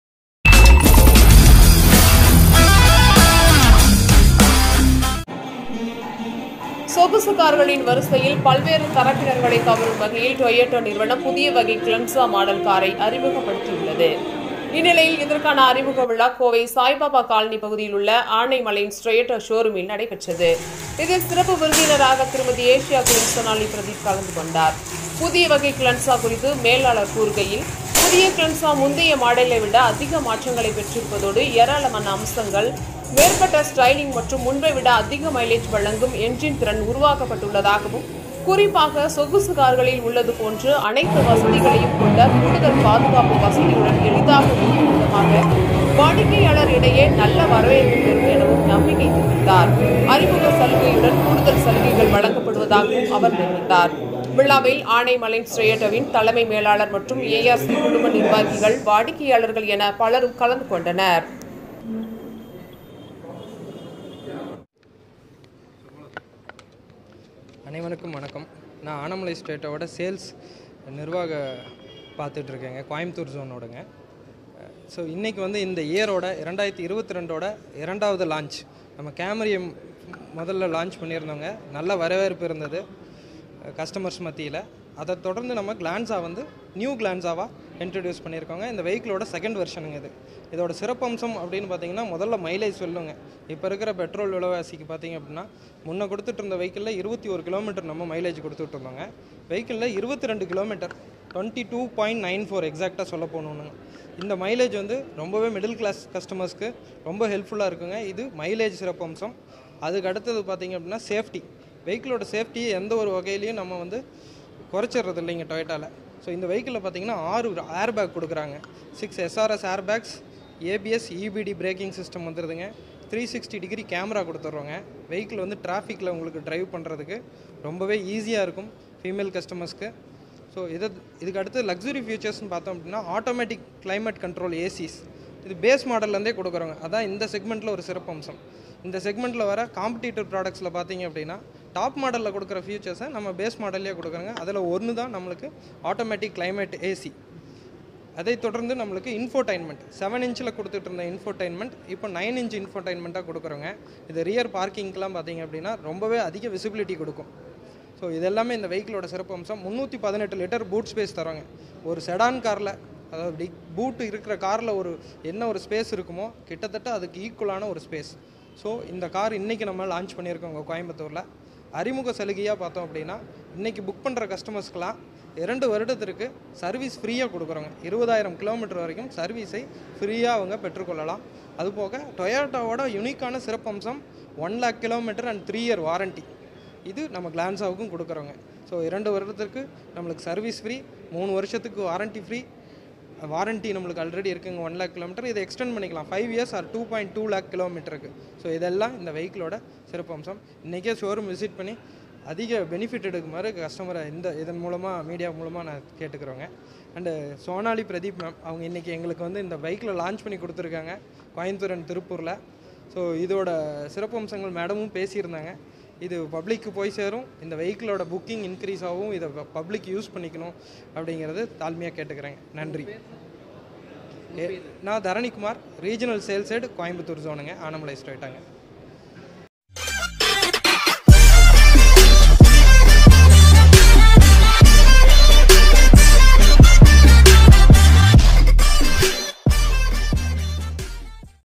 கோவை ஆனைமலைஸ் டொயோட்டா,ஷோரூமில் TOYOTA GLANZA வின் புதிய மாடல் ஹேட்ச்பேக் வாகன அறிமுக விழா
சொகுசு கார்களின் வரிசையில் பல்வேறு தரப்பினர்களை கவரும் வகையில் டொயோட்டா நிறுவனம் புதிய வகை க்ளான்சா மாடல் காரை அறிமுகப்படுத்தியுள்ளது..இந்நிலையில் இதற்கான அறிமுக விழா கோவை சாய்பாபாகாலனி பகுதியில் உள்ள ஆனைமலைஸ் டொயோட்டா ஷோரூமி்ல் நடைபெற்றது. இதில் சிறப்பு விருந்தினராக திருமதி ஏசியா குயின்  சோனாலி பிரதீப் கலந்து கொண்டார். புதிய வகை க்ளான்சா குறித்து மேலாளர் கூறுகையில், புதிய கிளான்சா முந்தைய மாடலை விட அதிக மாற்றங்களை பெற்று இருப்பதோடு, ஏராளமான அம்சங்கள், மேம்பட்ட ஸ்டைலிங் மற்றும் முன்பை விட அதிக மைலேஜ் வழங்கும் என்ஜின் திறனாக உருவாக்கப்பட்டுள்ளதாகவும்,குறிப்பாக சொகுசு கார்களில் உள்ளது போன்றே அனைத்து வசதிகளையும் கொண்டு,கூடுதல் பாதுகாப்பு வசதிகளுடன்,எளிதாக ஓட்டும் விதமாக வாடிக்கையாளர்களிடையே நல்ல வரவேற்பை பெறும் என நம்பிக்கை தெரிவித்தார்..அறிமுக சலுகையுடன்,கூடுதல் சலுகைகள் வழங்கப்படுவதாகவும் அவர் தெரிவித்தார்..விழாவில்,ஆனைமலைஸ் டொயோட்டாவின் தலைமை மேலாளர்கள் மற்றும் ARC குழும நிர்வாகிகள் ,வாடிக்கையாளர்கள் என பலர் கலந்து கொண்டனர்.